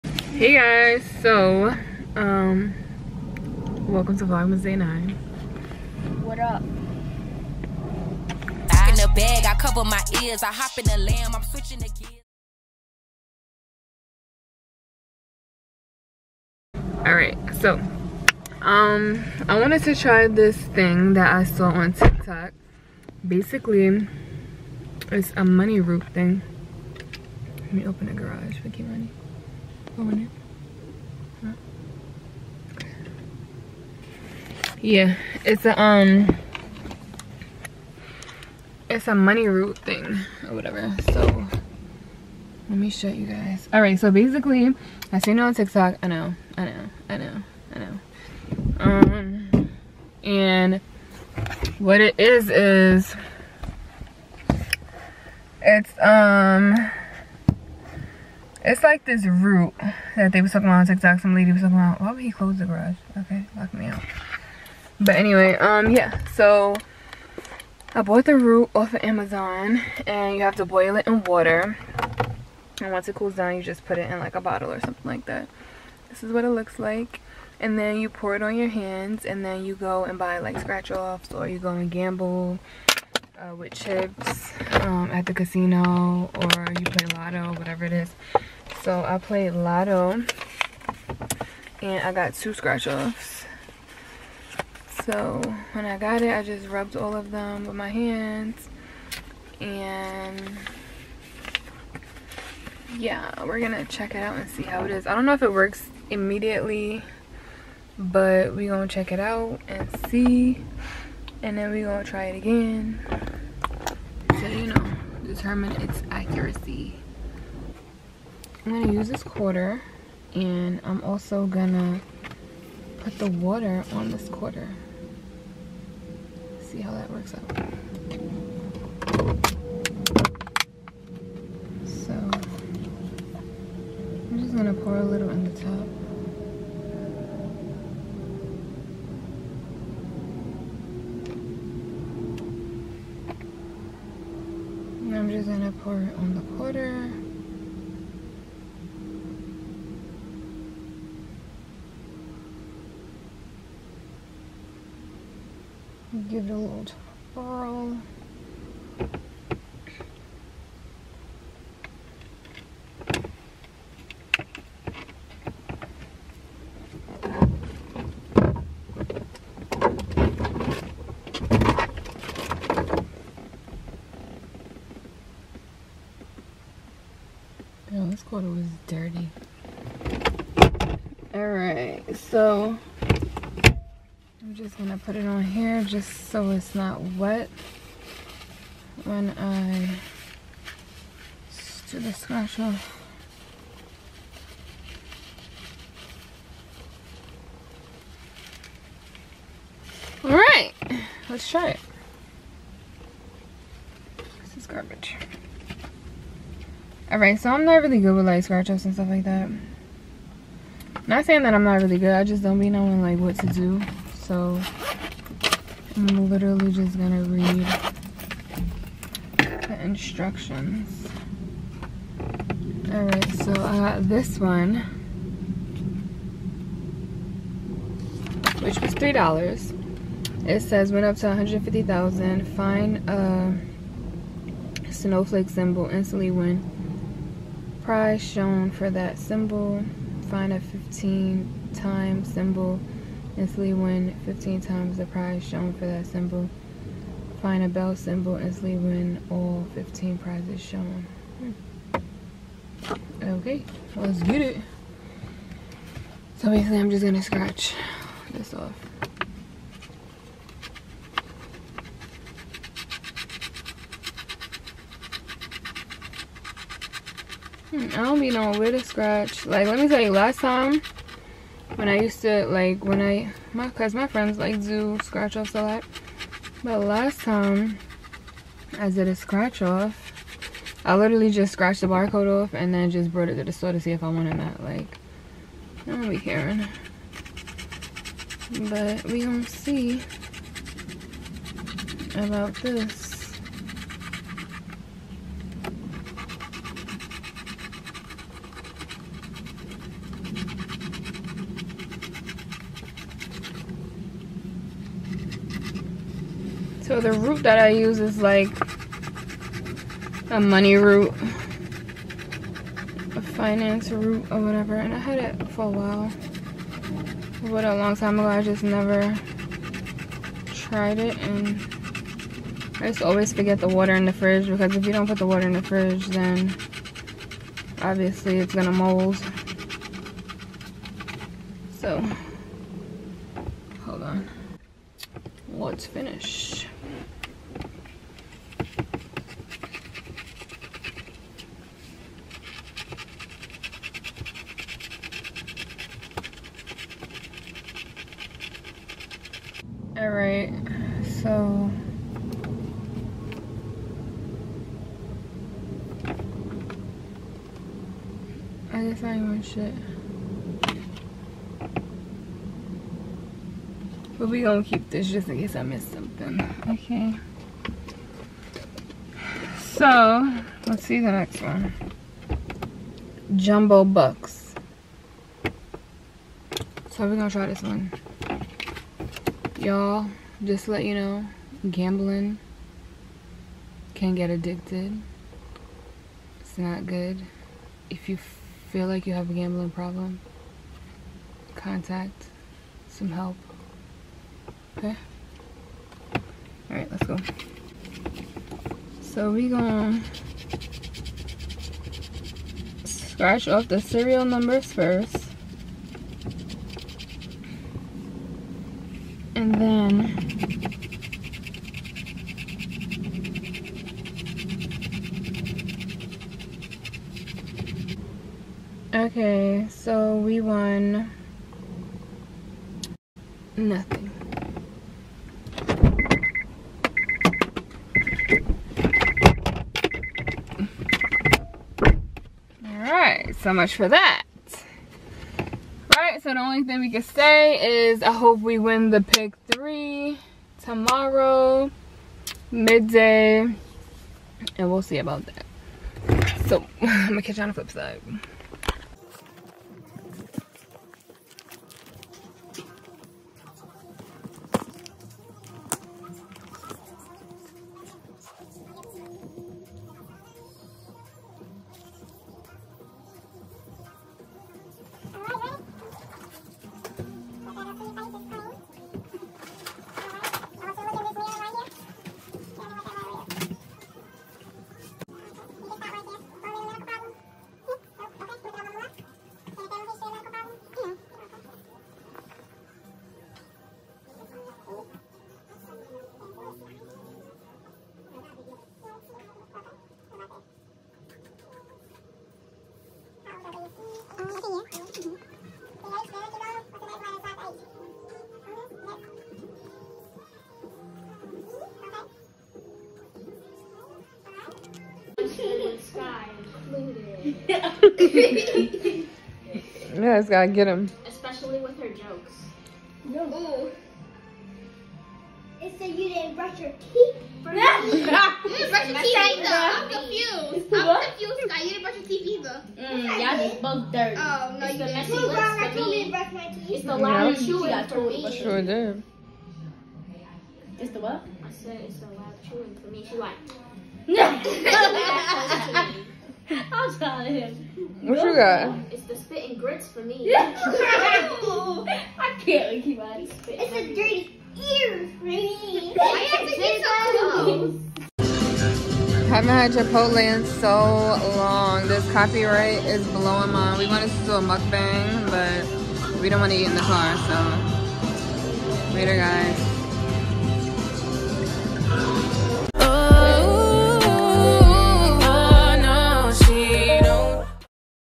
VLOGMAS DAY #9 | Money Root | by. Malone
It’s Vlogmas DAY #9
Money root
Mani & Skai go at it…
I will be uploading every single day in December!

M Squaaaaaaad! (In my Brooklyn voice lol)